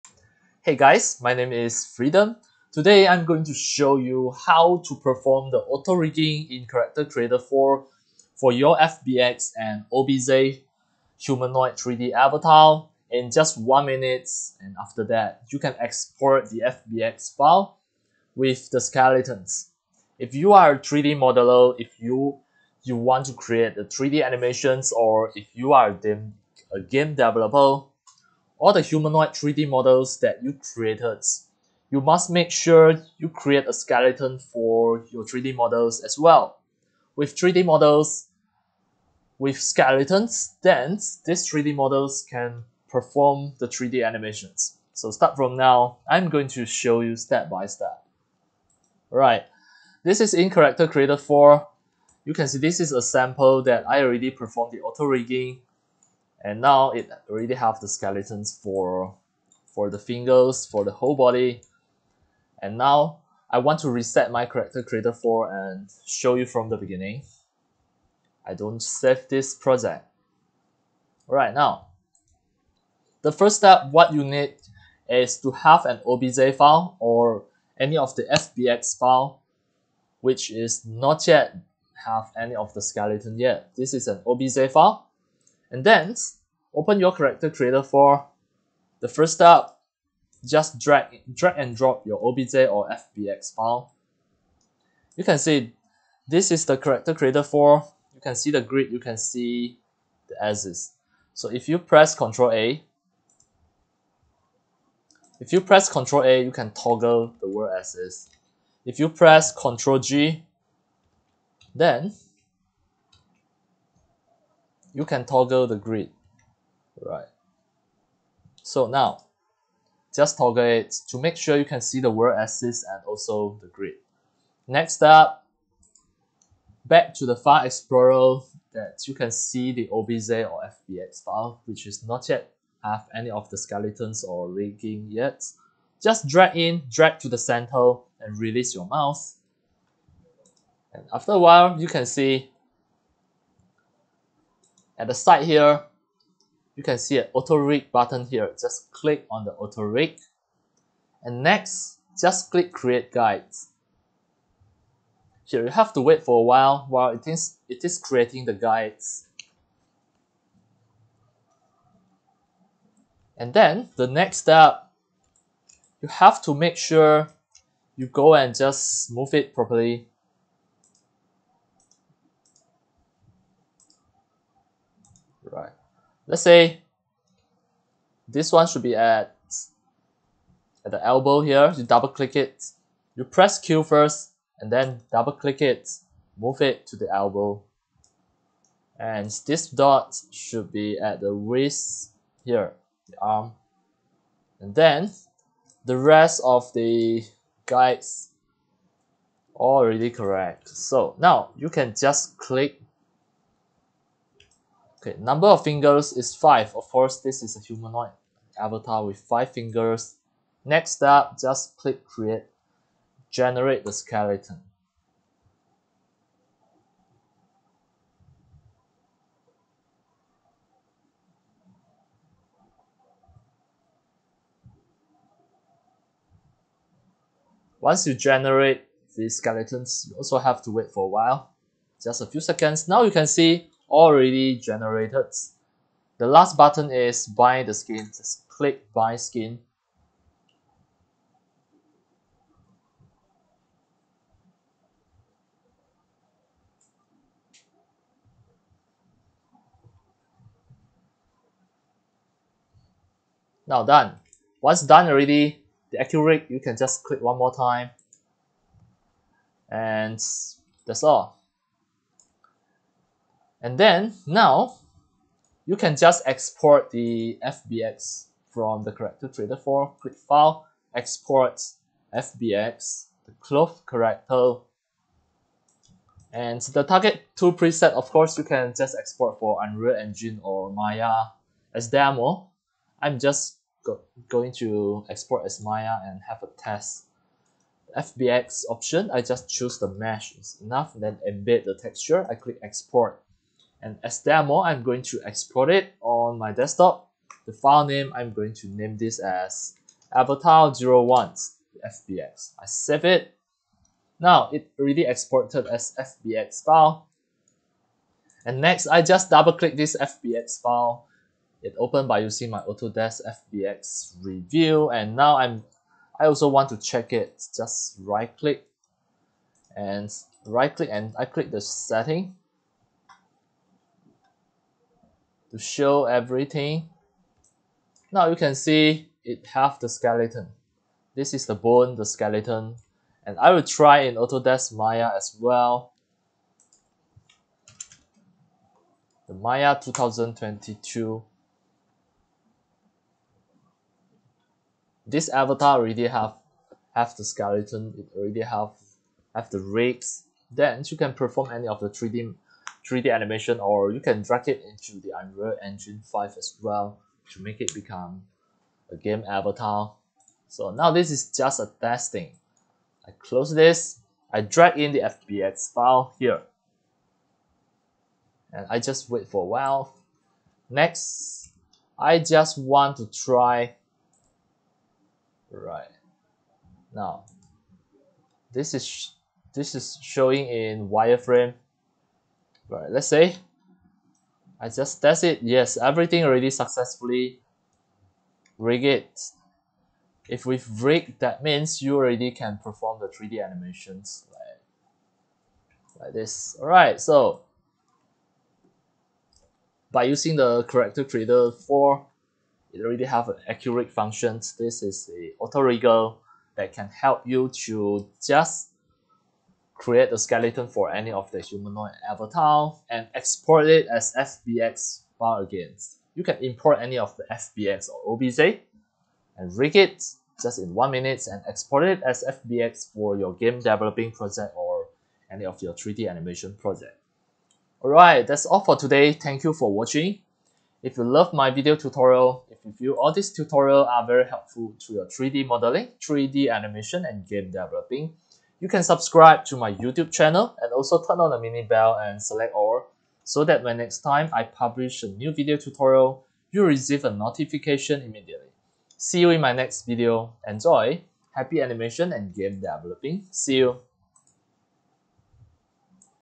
AutoRigger for FBX & OBJ in 1-minute - Character Creator 4 Accurig Tutorial
- Hey guys, how to auto rig your FBX and OBJ humannoid 3D model in 1-minute? The latest update of Character Creator 4.1 has an AutoRig function, you can import your FBX/OBJ, autorig it, and export as FBX with skeleton! This is my step-by-step tutorial on how to do this! ENJOY! and have fun! Happy 3D Modeling, 3D Animation and 3D Game Dev! ENJOY!

Get iClone 8 and CC4 Contents

Your friend,
FREEDOM